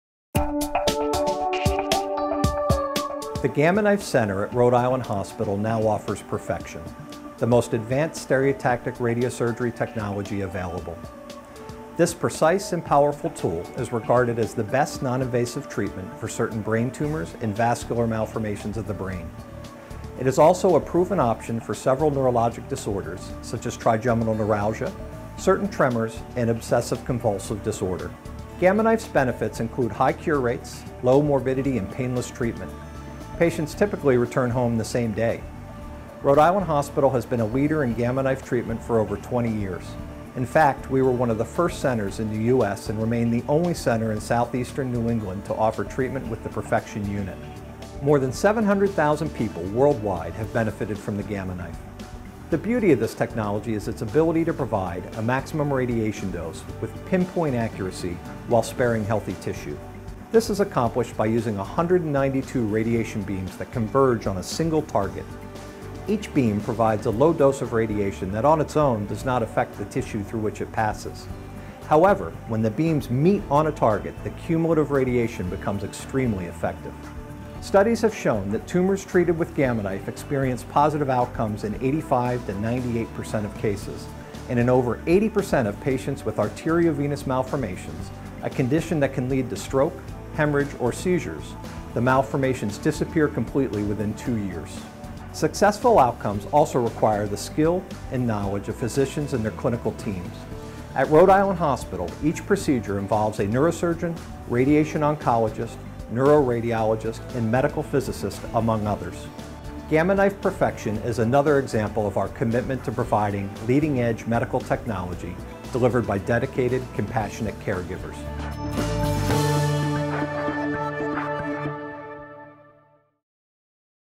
Gamma Knife at Rhode Island Hospital
Rhode Island Hospital’s Gamma Knife Center brings the power of “Perfexion” to patients. This new technology offers an enhanced level of precision and care for certain types of brain tumors or vascular malformations of the brain.